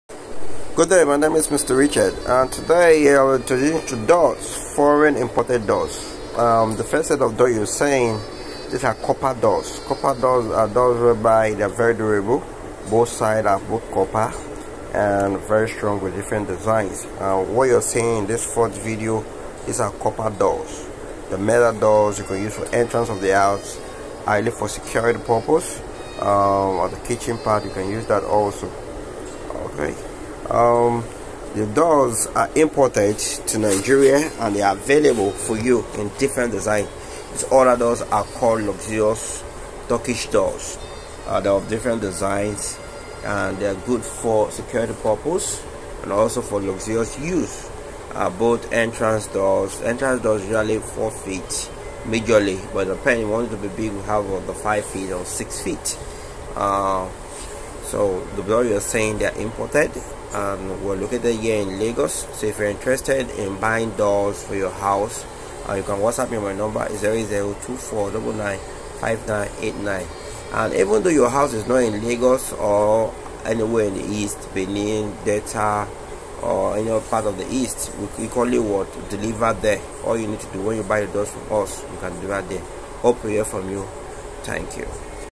Doors imported to Nigeria for sale at Lagos Nigeria 08024995989 call or Whatsapp
imported doors for sale in Lagos
interested in buying Turkish doors

we are located in Lagos Nigeria .

we can supply and deliver in any location even out side Lagos .